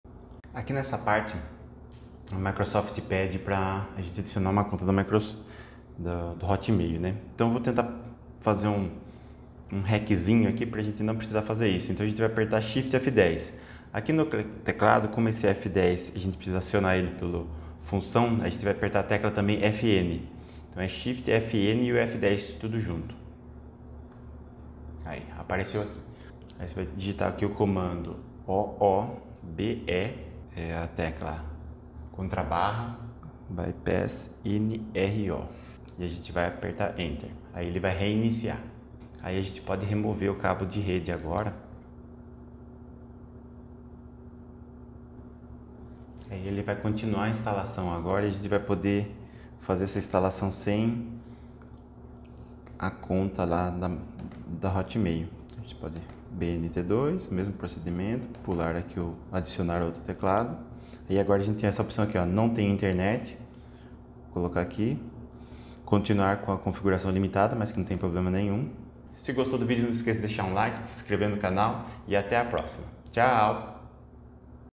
Como Instalar o Windows Sem Uma Conta Microsoft
Quer aprender a instalar o Windows sem precisar de uma conta Microsoft? Este vídeo é para você! Neste tutorial prático, revelo o passo a passo para instalar o Windows sem a necessidade de uma conta Microsoft.
Assista agora e domine essa habilidade essencial para configurar seu Windows do seu jeito, sem amarras! Deixe seus comentários com dúvidas ou sugestões para futuros vídeos. Vamos juntos simplificar o mundo da informática! 💻🔧